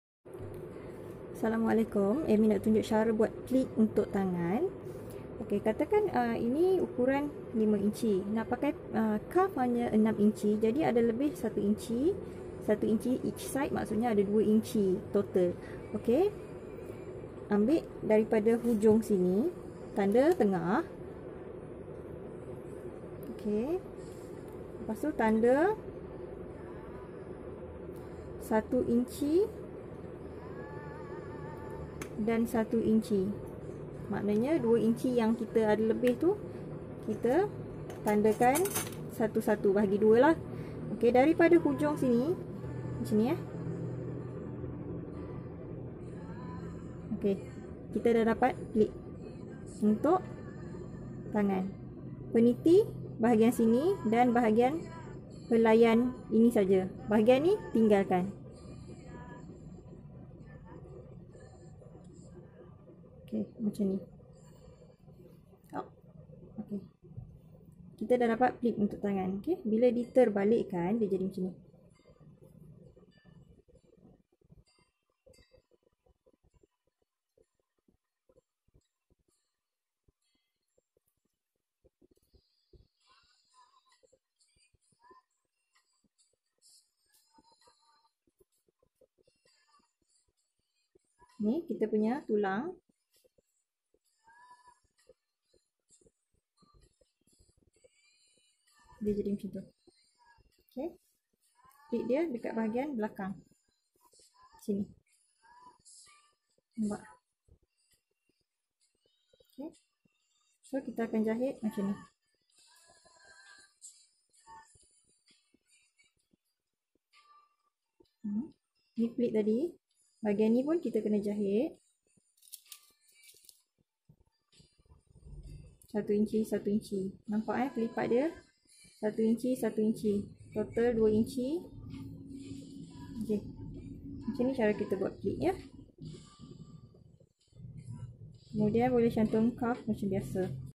cara pleat bukaan lengan kemeja.
video ini memaparkan cara membuat pleated lengan baju kemeja lelaki.